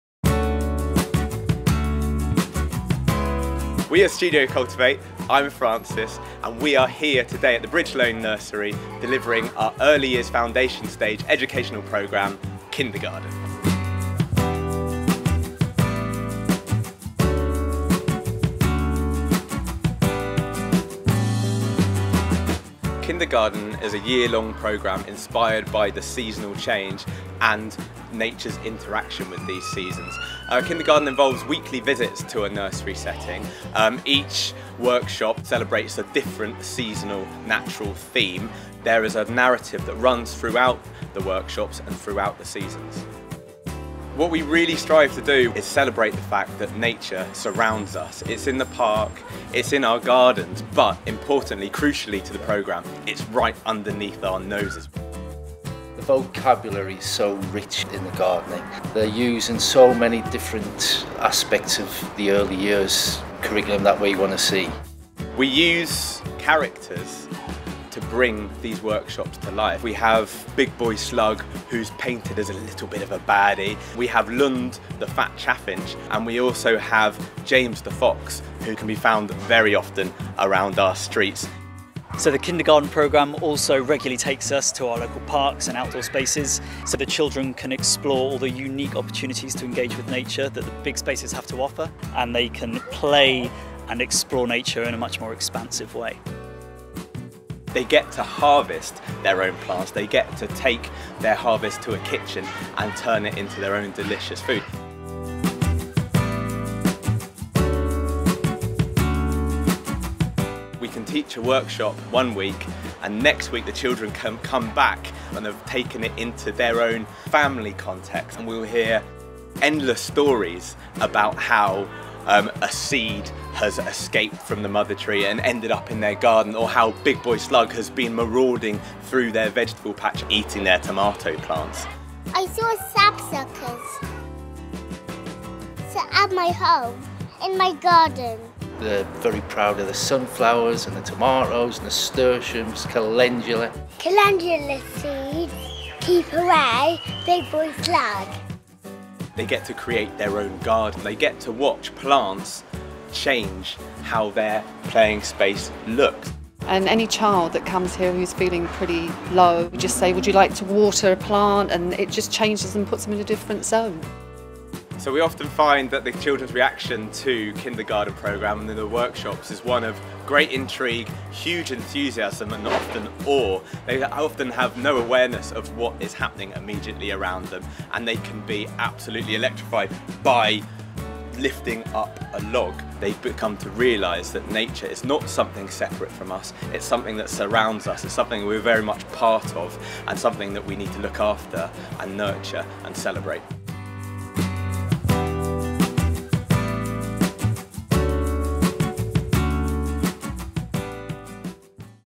Studio Cultivate presents Kindergarden
Kindergarden is Studio Cultivate's early-years programme that engages young children with the wonderful world of nature through exciting weekly workshops.

Filmed by Joseph Edwards.
Thanks to Bridge Lane Nursery.